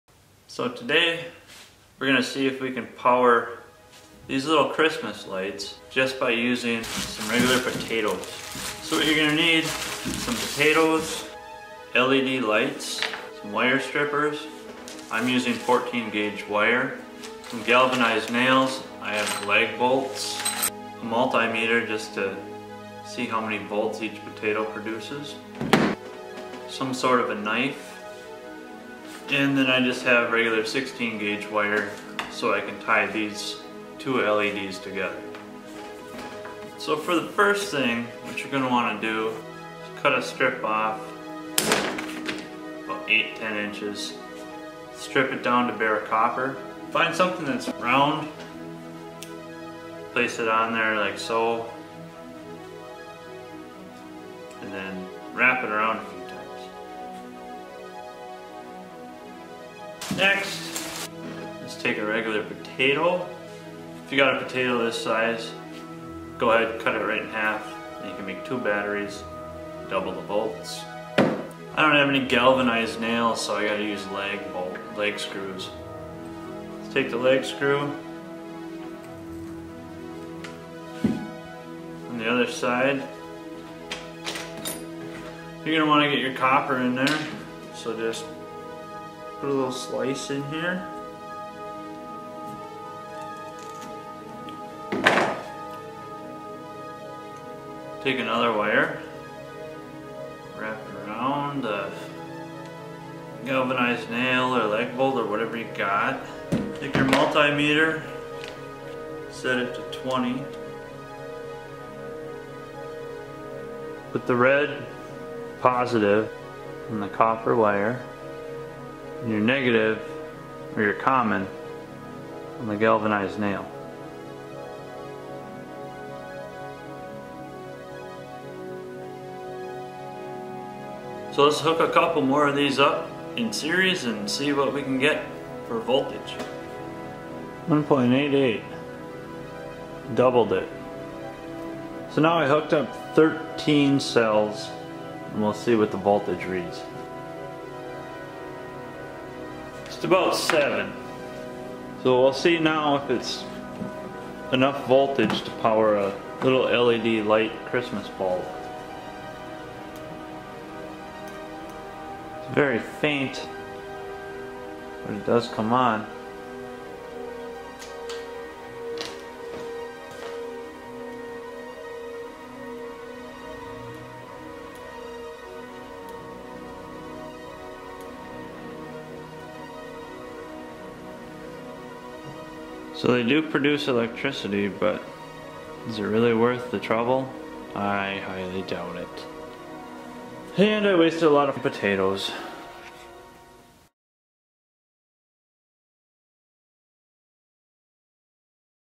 Potato light bulb experiment for kids
I will show you how to harness electricity from some ordinary household goods. By taking a few potatoes, some regular copper wire and some galvanized nails I demonstrate the power of a potato. One potato nearly reveals .9 VOLTS and while hooking a few together in series will harness even more. Enough to power an LED bulb.